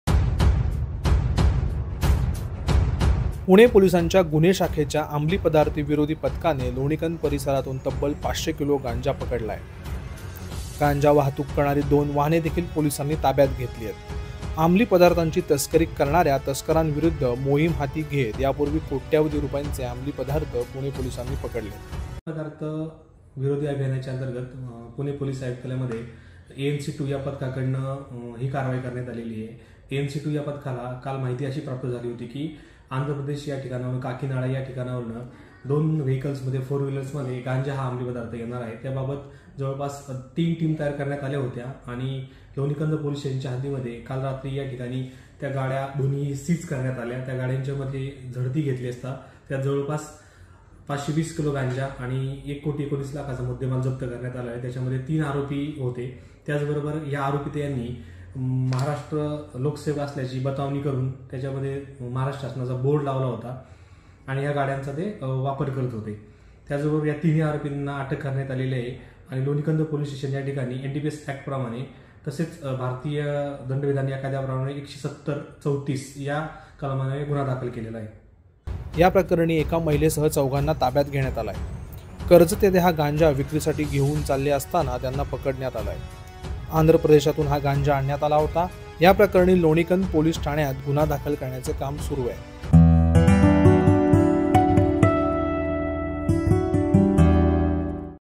Pune | आंध्र प्रदेशातून आणलेला पाचशे किलो गांजा पुण्यात जप्त... | Digital Prabhat
Pune | आंध्र प्रदेशातून आणलेला पाचशे किलो गांजा पुण्यात जप्त...